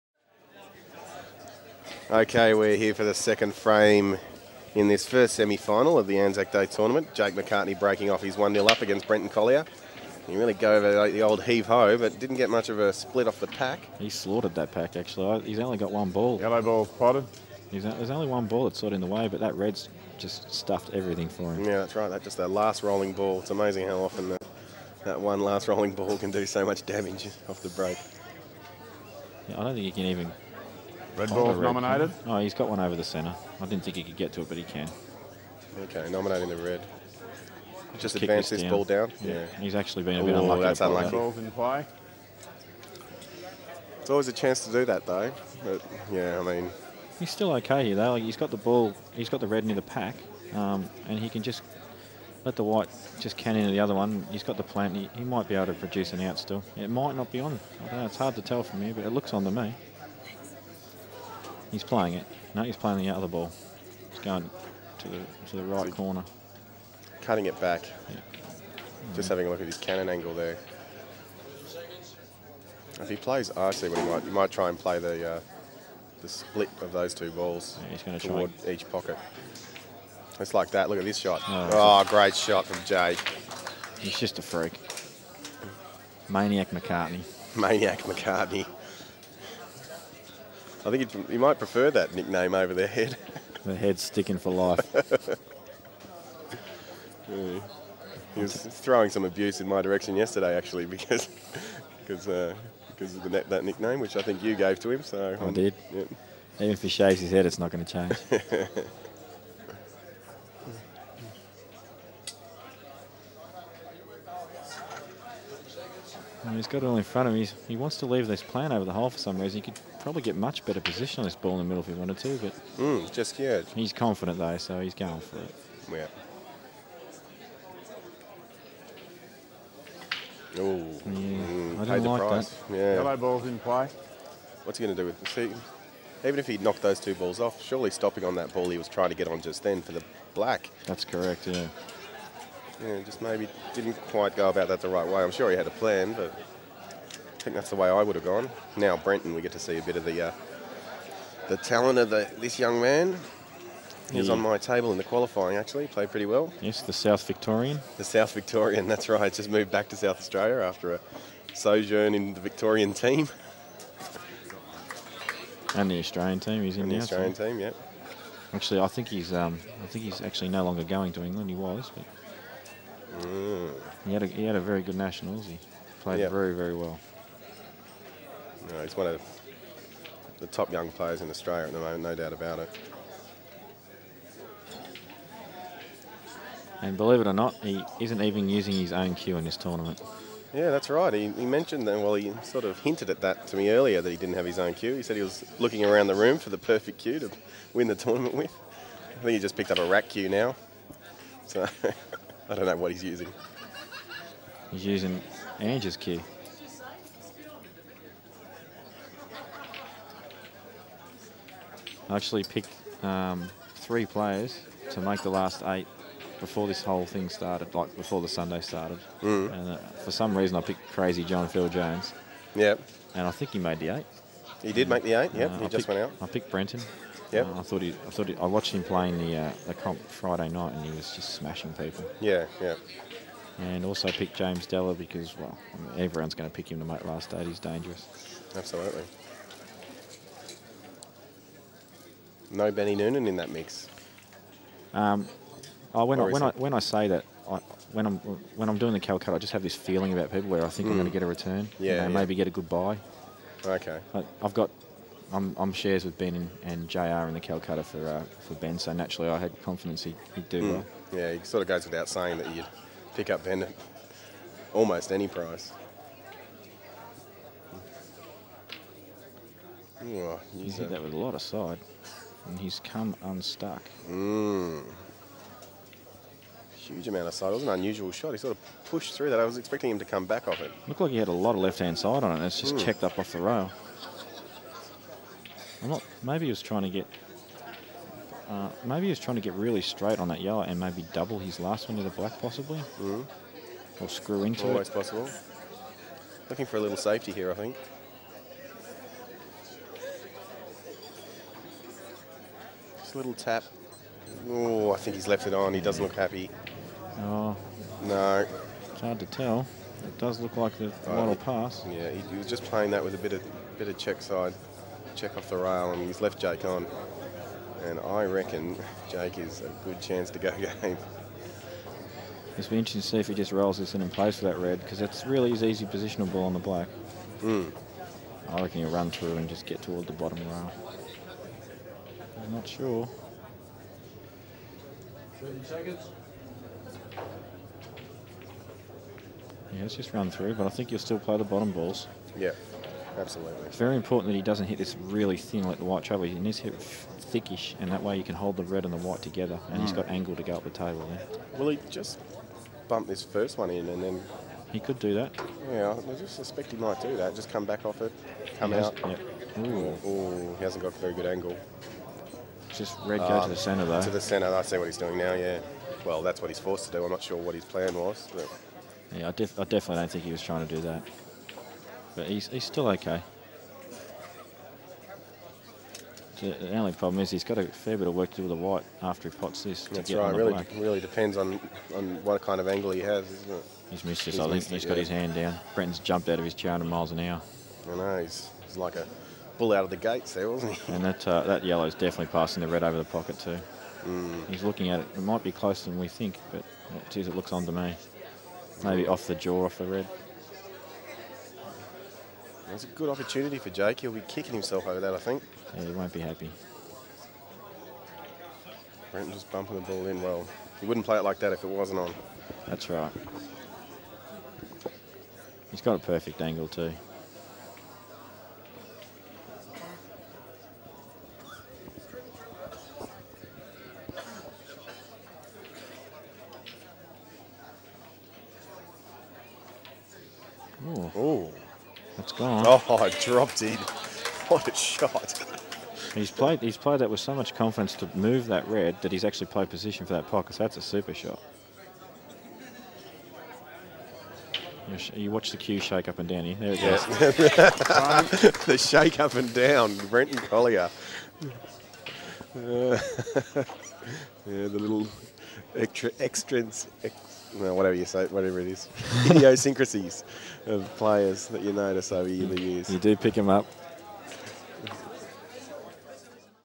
8BallTV Jake McCartney Vs Brenton Collier Semi Finals FRAME 2 ANZAC 2007
anzac day 8 ball championships 2007
Jake and brenton